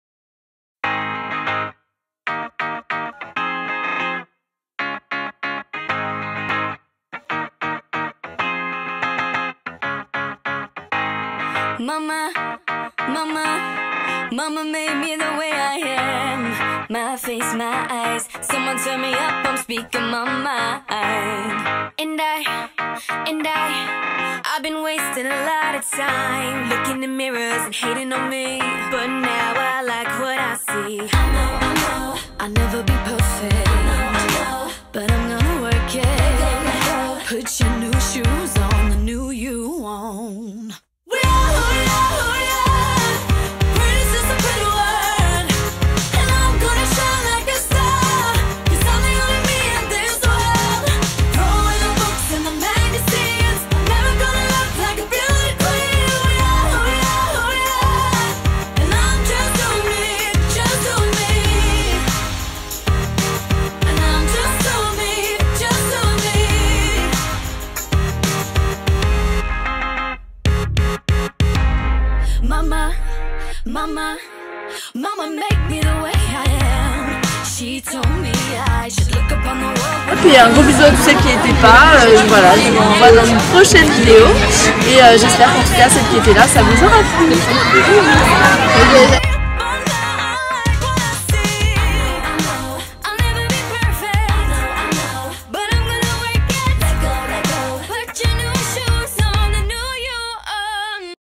BIG Meet-up ! (29/08/2013)
Superbe Meet-up  organisé par Léa (Jenesuispasjolie),Jeanne (LittleJBeauty),
Amélie (itsmyamelie) etc.

Je filme avec un Panasonic SDR-S7
Je fais mes montages avec IMovie (uniquement sur Mac)